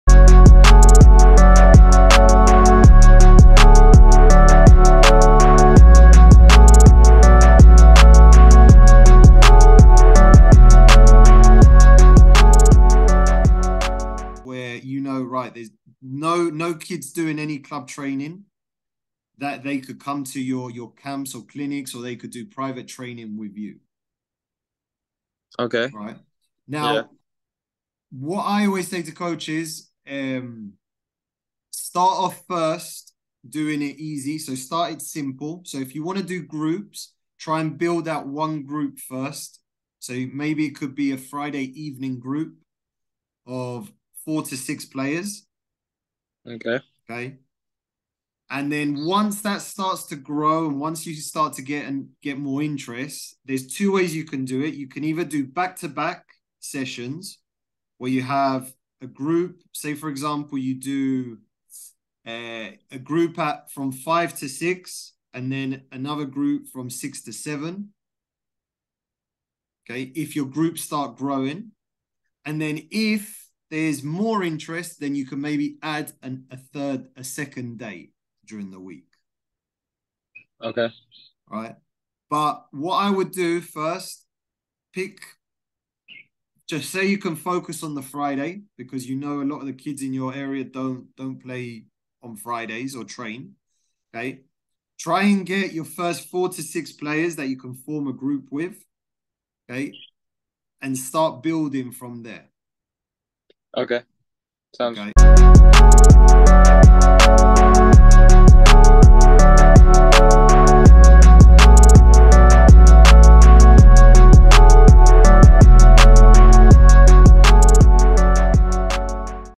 ⚽︎ Launch Your Own Soccer Academy - Ultimate Guide to Small Group Training!
In today's video, you will learn how to build a small group football coaching business in your city.